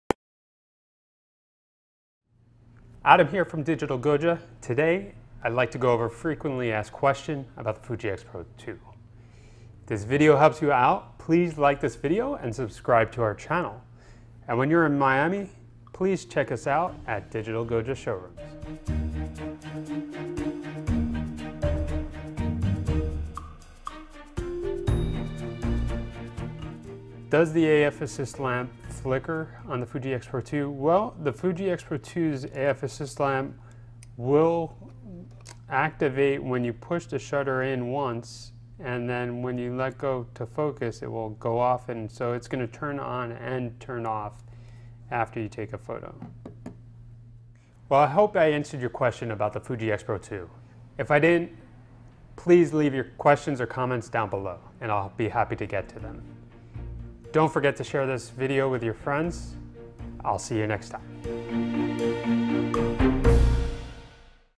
Does the Fujifilm X-Pro2 AF Assist Lamp Flicker? | FAQ
We test the Fujifilm X-Pro2 to find out if the AF assist lamp is supposed to flicker! (Find yours CommissionsEarned) Find out the answer by watching this video.

Features of the Fujifilm X-Pro2:
- 24.3MP APS-C X-Trans CMOS III Sensor
- X-Processor Pro Engine
- Advanced Hybrid Multi Viewfinder
- 3.0" 1.62m-Dot LCD Monitor
- Full HD 1080p Video Recording at 60 fps
- Built-In Wi-Fi, SHARE Printer Compatible
- 273-Point AF with 77 Phase-Detect Points
- Up to 8 fps Shooting and ISO 51200
- Weather-Sealed Design, 2x SD Card Slots
- Film Simulation and Grain Effect Modes

Fujifilm X-Pro2 FAQ | Is the AF Assist Lamp Supposed to Flicker?